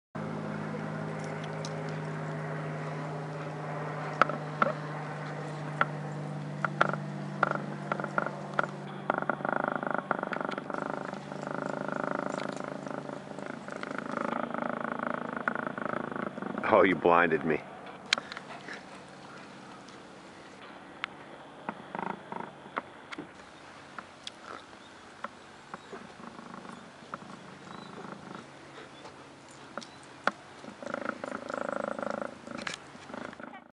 glimpse of Ones heeling
Just a taste of what you can get from the Fundamental's 2 and the heeling class at the Fenzi academy